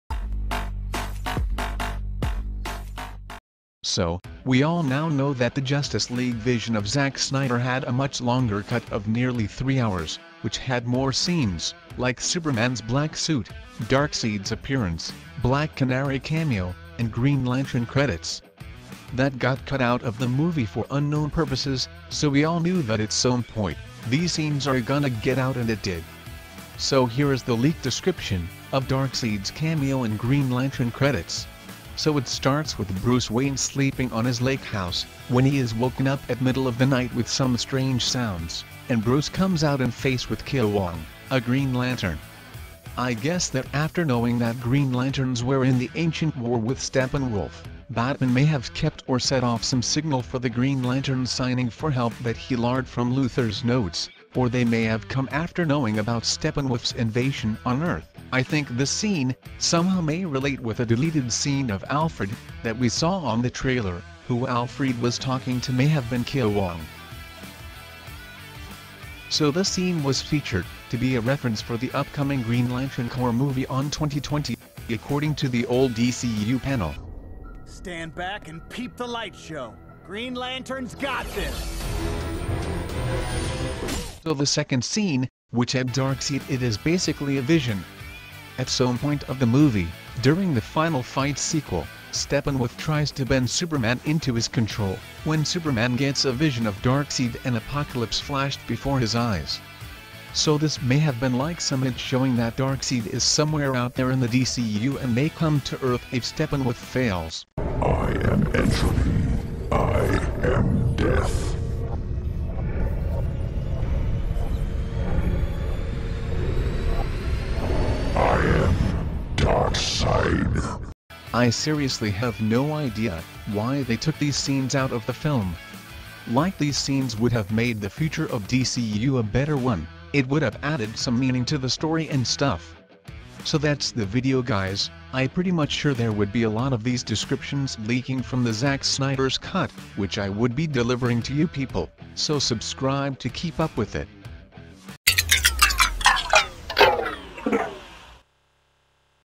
Leaked Green Lantern & Darkseid Deleted Scene | Justice League | Descriptions
So these are the leaked description of the deleted scenes of justice league's zack snyder cut.